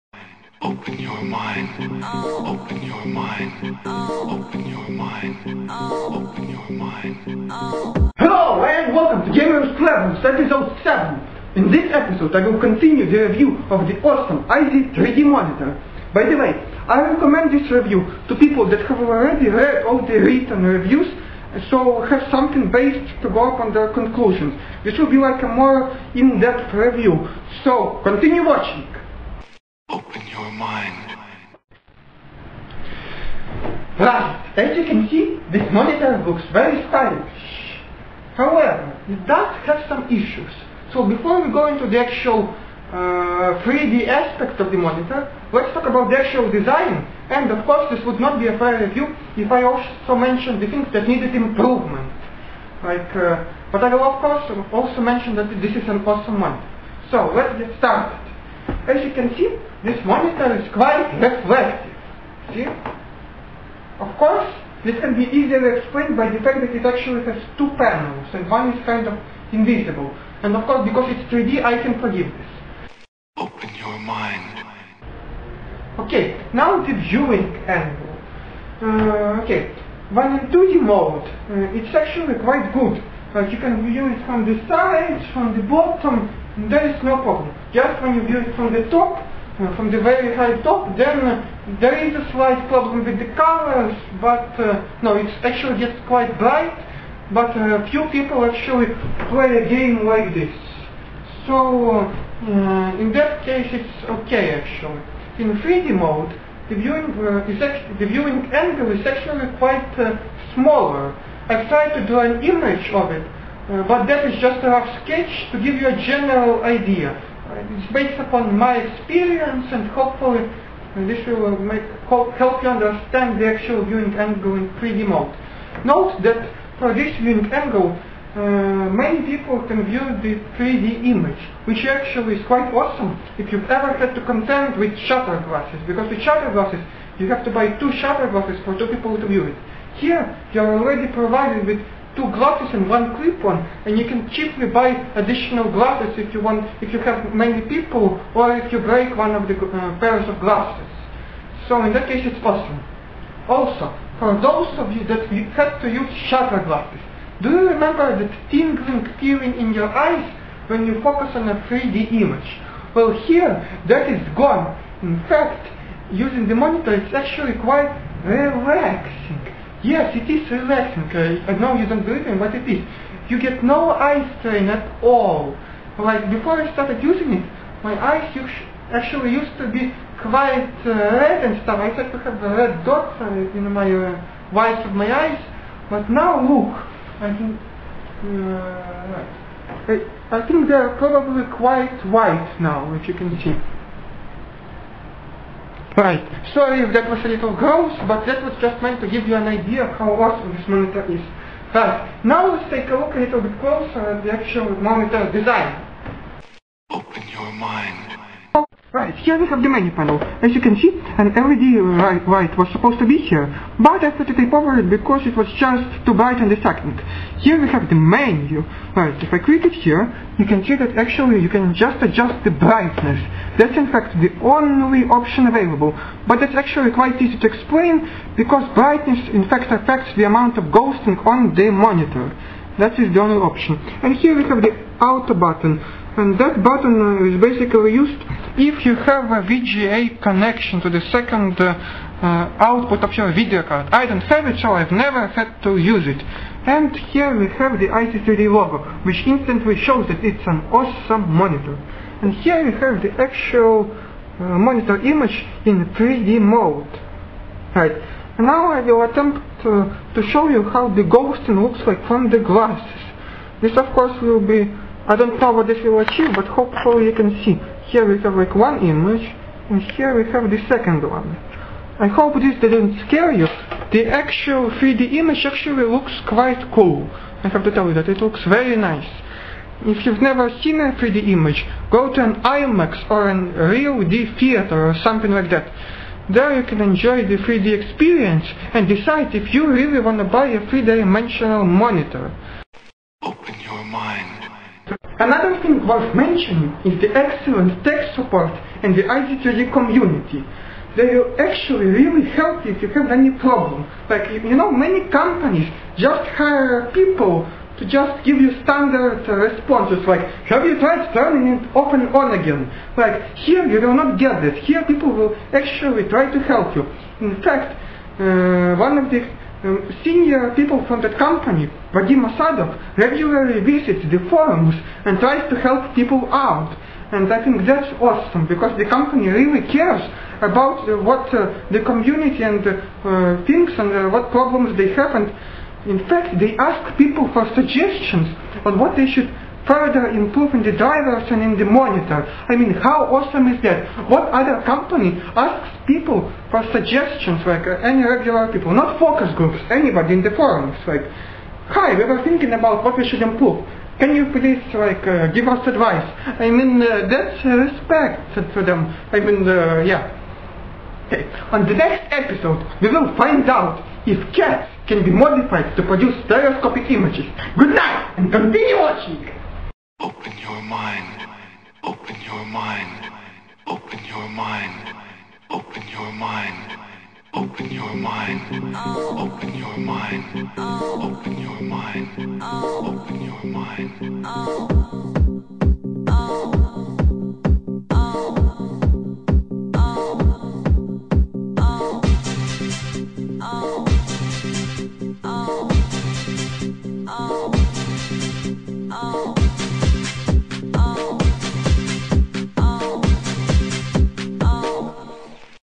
GC ep7: iz3d review part 2
In this episode I continue my review of the iz3d monitor. Also in one part where I do a close up of it I don't think I really spoke too clearly. In any case there I say that the LED light thingy on the monitor is too bright and an option to turn it on/off would be nice so I scotched taped some cardboard to it.